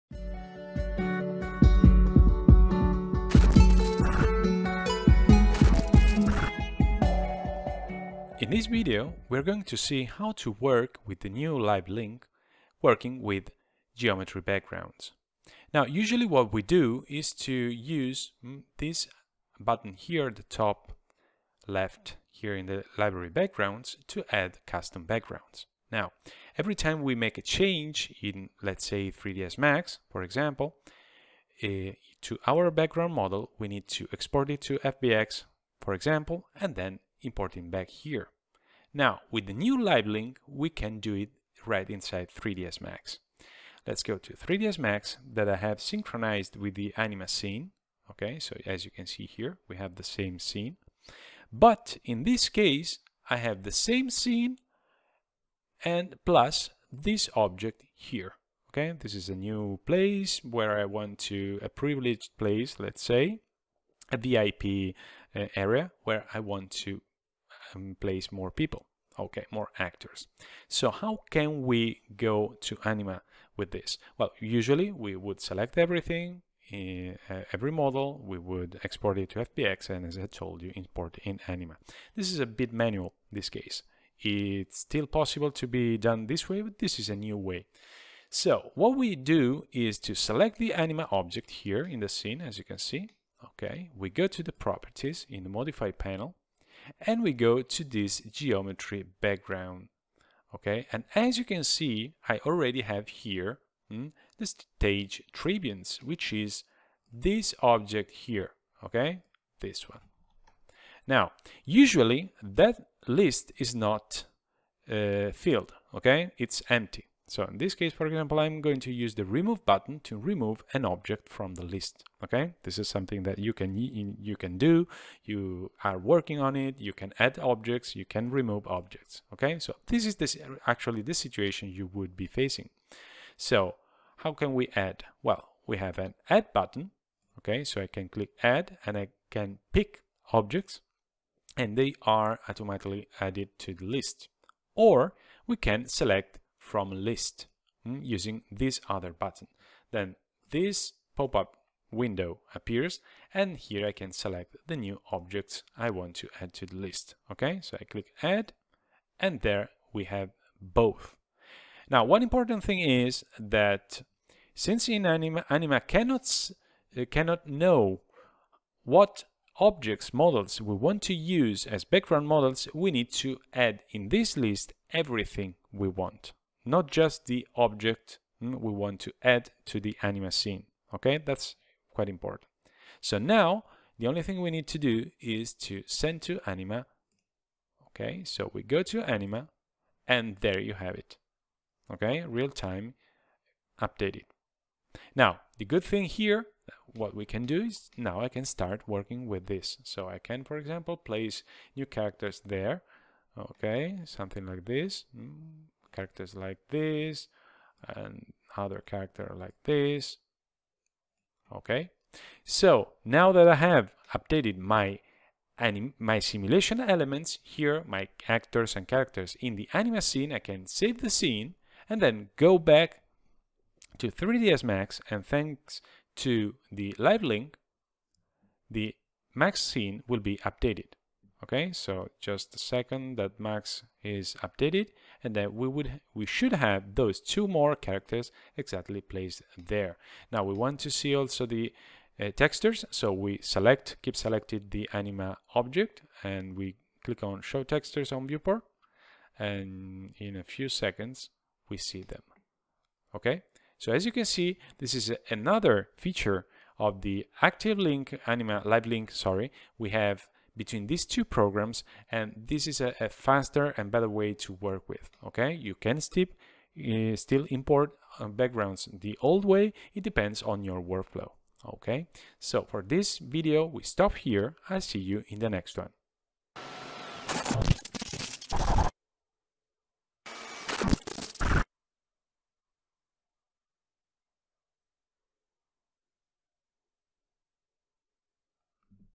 5.0.5 - anima® 3.5 Synchronizing Geometries with the new alive® link system
Define and select the collision geometries for your simulation directly in 3ds Max® and send them to anima® within a click!